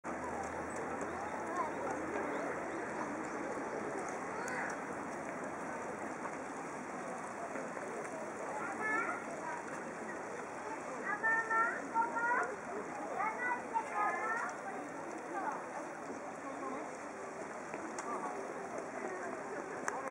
佐賀県多久市 「 西渓公園 」
西渓公園は、炭坑王・高取伊好が大正９年から１３年にかけて、巨額の私財を投じて整備し、当時の多久村に寄贈した公園である。